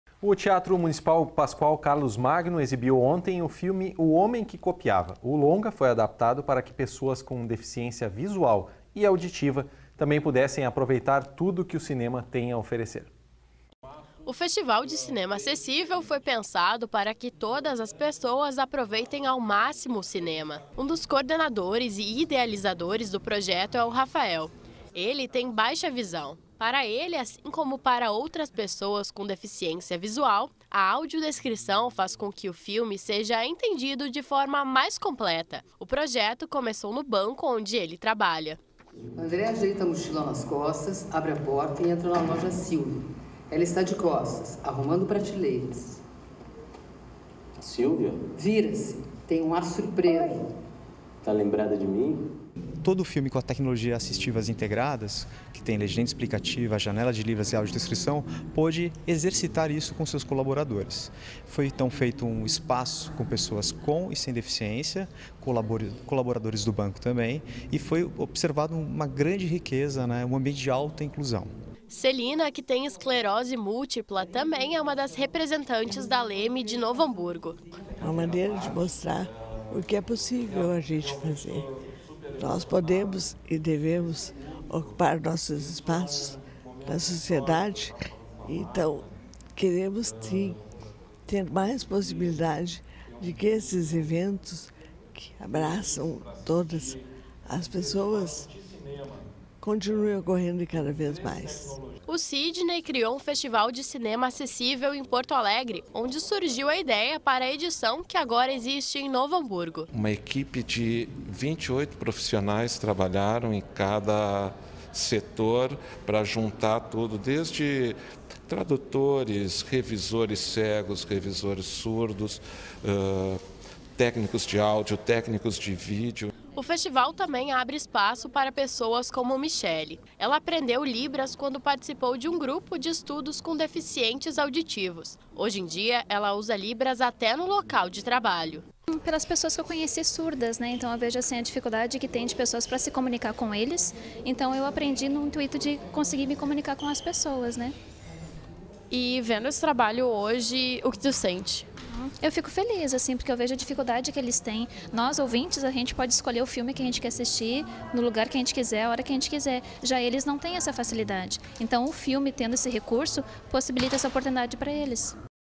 TV Feevale Notícias - Festival de Cinema Acessível
O Teatro Municipal Paschoal Carlos Magno exibiu ontem (19) o filme O Homem que Copiava. O longa foi adaptado para que pessoas com deficiência visual e auditiva também pudessem aproveitar tudo que o cinema tem a oferecer.